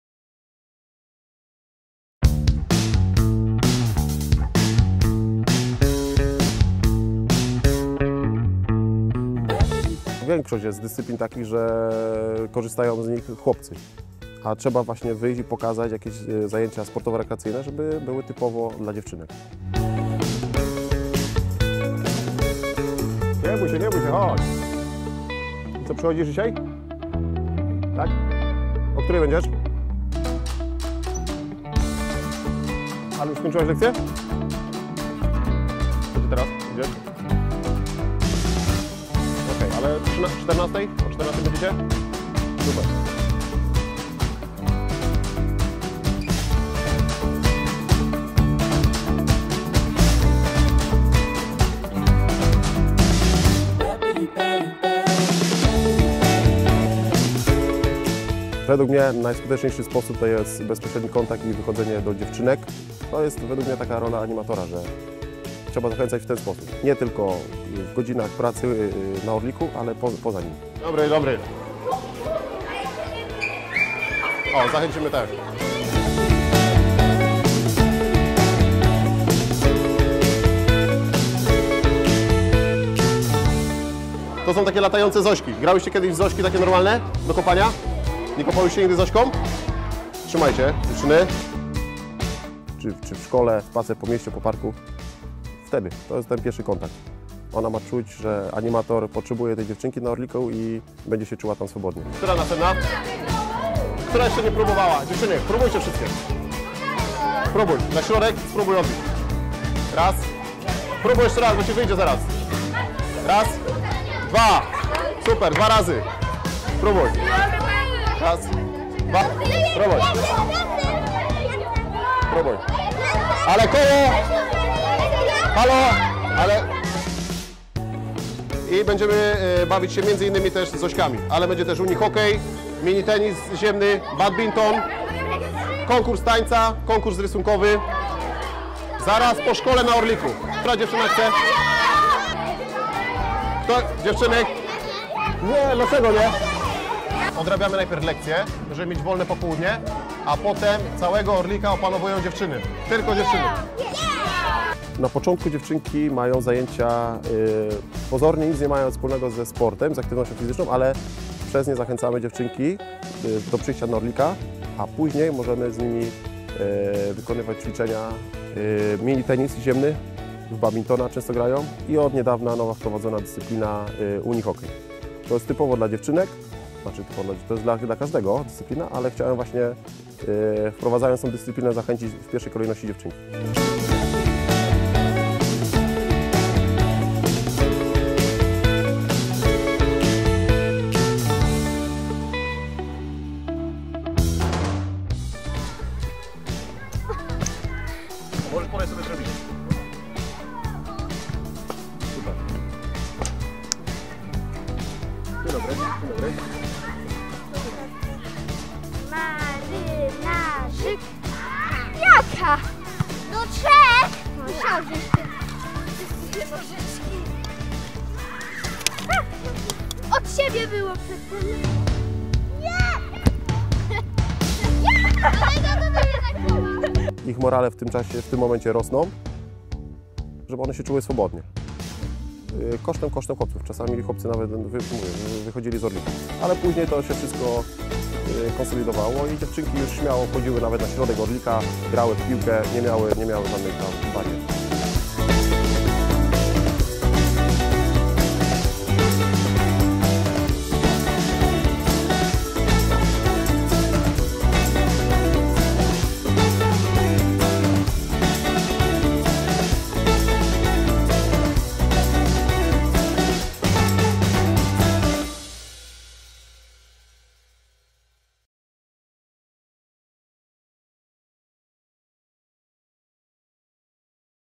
Jak pracować z dziewczynkami na orliku - animator Jakub Zmyślony
Jakub Zmyślony pracuje jako animator na orliku w Sycowie. Ma duże doświadczenie w prowadzeniu zajęć dla dziewczynek. Pokazuje, jak wykorzystywać różne okazje, by zachęcić dziewczęta do przyjścia na orlik.

Film powstał w ramach projektu Akademii Animatora 2014 finansowanego przez Ministerstwo Sportu i Turystyki.